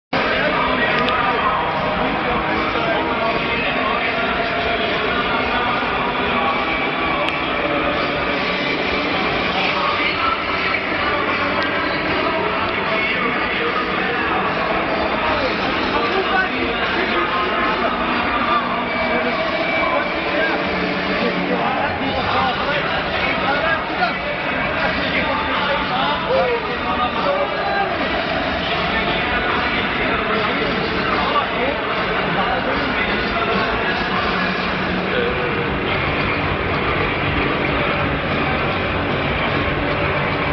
View from My Seat at Celtic V Ac Milan
the View I got from my seat Pre Match at celtic v Ac Milan in october 07 zooming in on players training and the seats of the members of the Upper Nithsdale celtic supporters club lol!